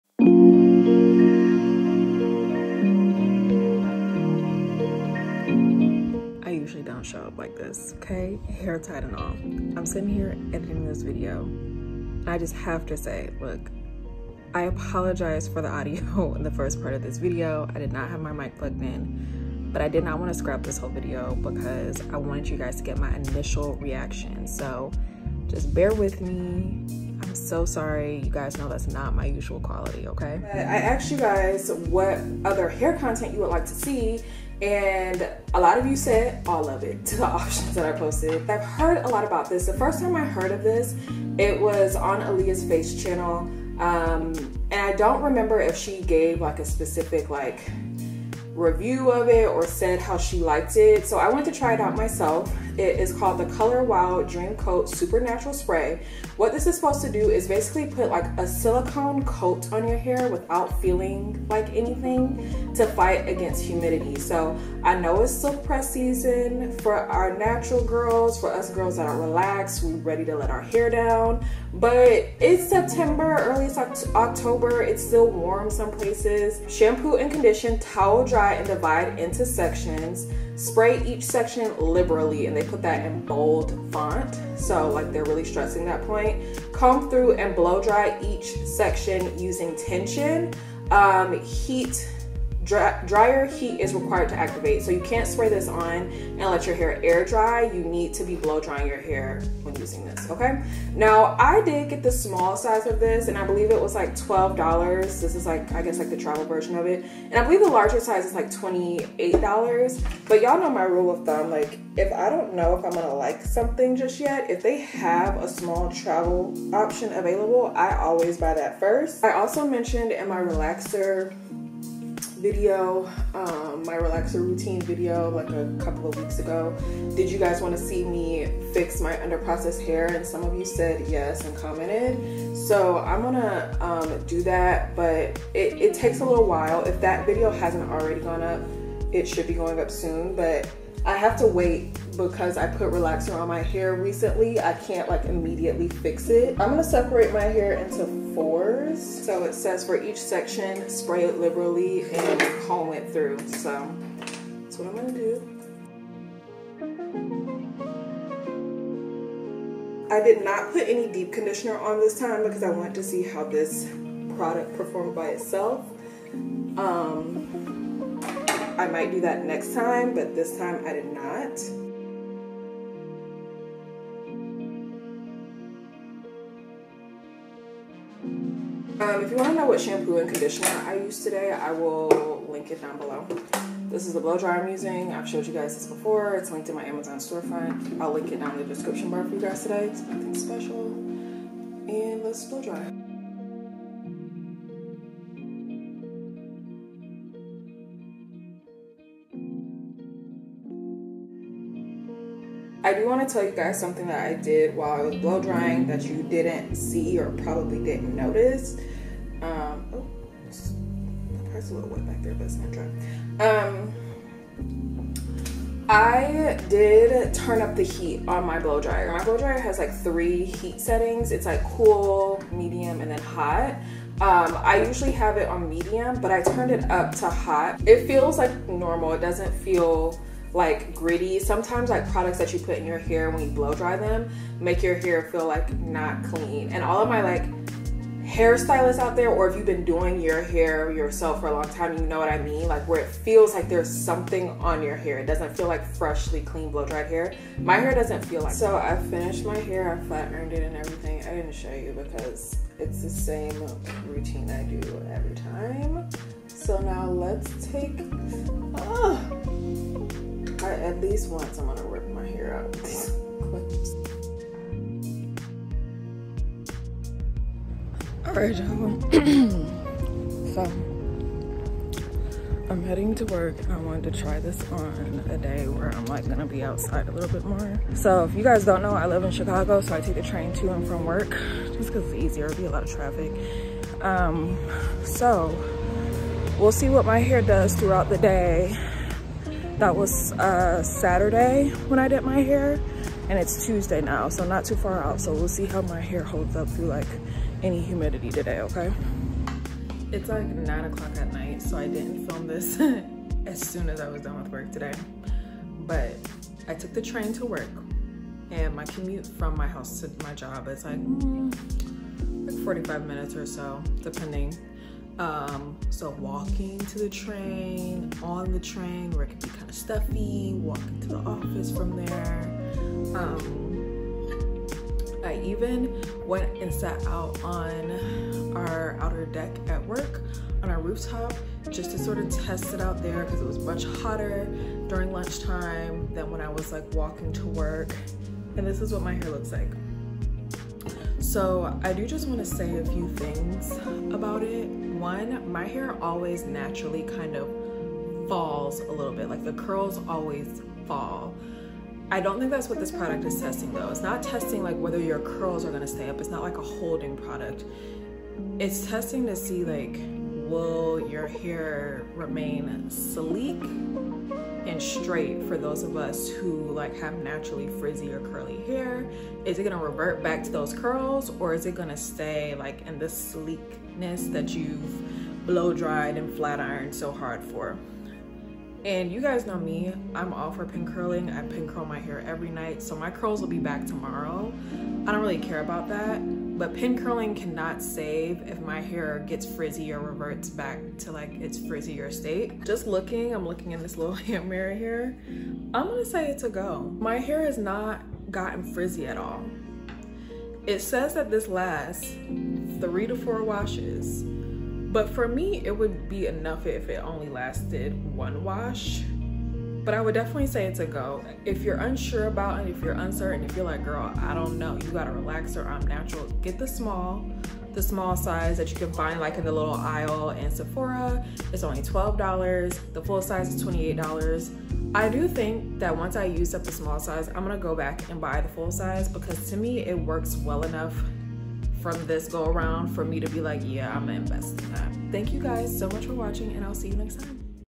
Color Wow Dream Coat Spray Humidity Test & Review | Does it work for textured hair? | Niara Alexis
Bookmarks:
Instructions 1:16
Price 1:39
Application 2:30
Humidity Test 4:35
Humidity Test Results 5:05